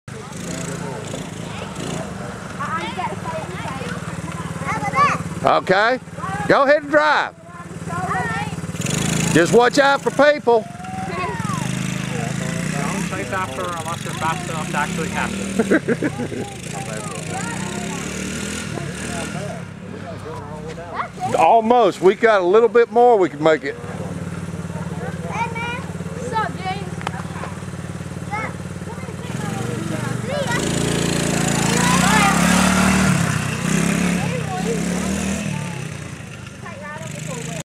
MOV02B.MOD
My grandson Wil s 7th birthday party riding his birthday present.